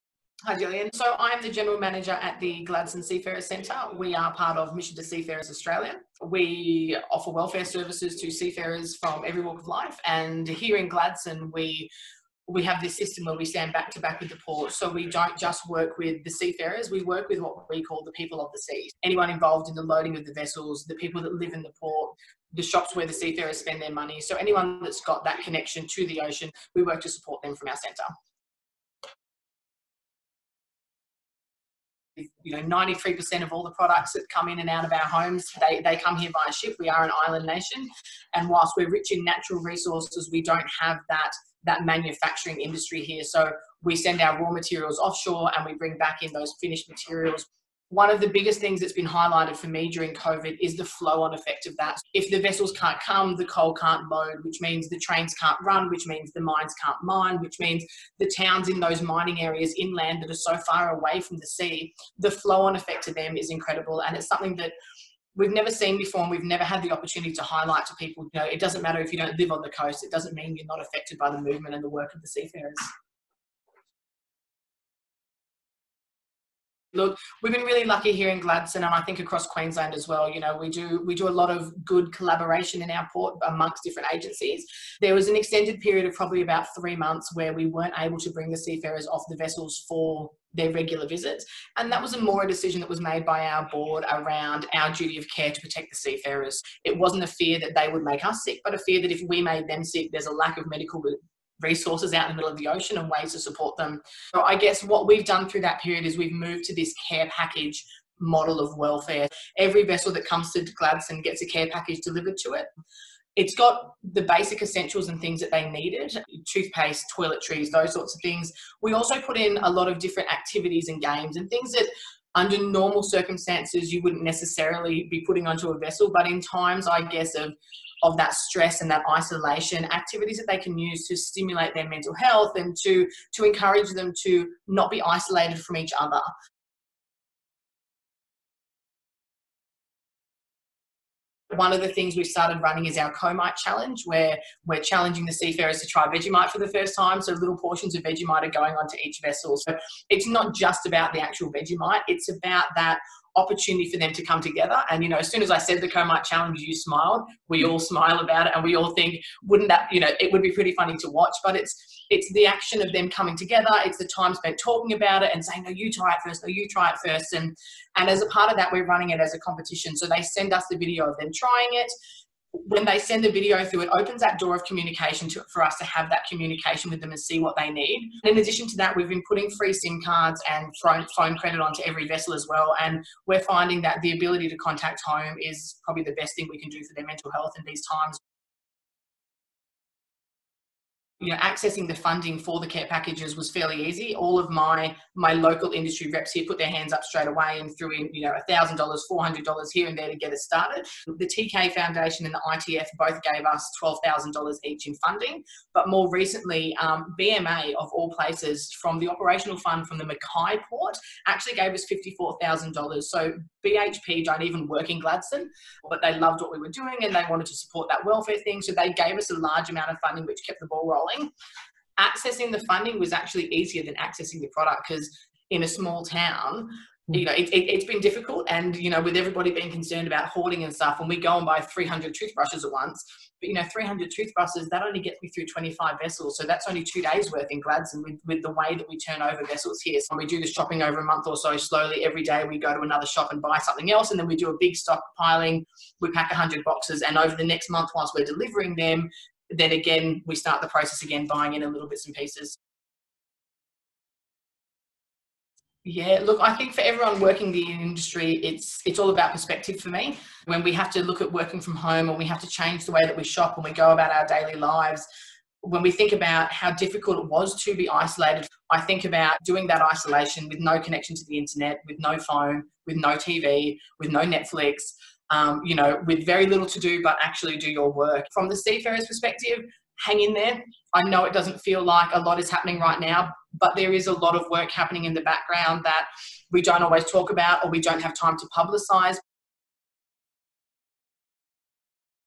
Maritime Matters – the Maritime Professional during covid19 – meet Jessica Mulhall
Welcome back – today we have a chance to catch up with Jess Mulhall, the Manager at the Gladstone Seafarers Centre.  Jess has worked hard to keep the energy of the centre up over the past months, through the full lock-down and now into the limited shore leave.
While covid has been, and still is, difficult, it provides an opportunity to highlight to the wider community the importance of the maritime industry.
To support both the physical and mental health of seafarers, the Gladstone centre has been packing and delivering boxes with basic supplies and activities.  This includes the Comite Challenge – an opportunity to try Vegemite (I say yuck to that!).  But the challenge isn’t about the Vegemite, its about the opportunity to connect, and to open avenues of communication.
Looking to the coming weeks it is important to keep perspective – both for those ashore and afloat.  Sometimes the best perspective is to see what you can do to help – check out your local seafarer welfare centre and find out what they need.  For all the seafarers, know that – while it may not seem like it, there are many people and organisations working hard to see what can be done to support all you do.
To all those working in the maritime, thank you - your role is critical, you are keyworkers.
What can you do to help the seafarers? Do you know where the local seafarer welfare centre is?  Might be worthwhile to take a moment to look it up!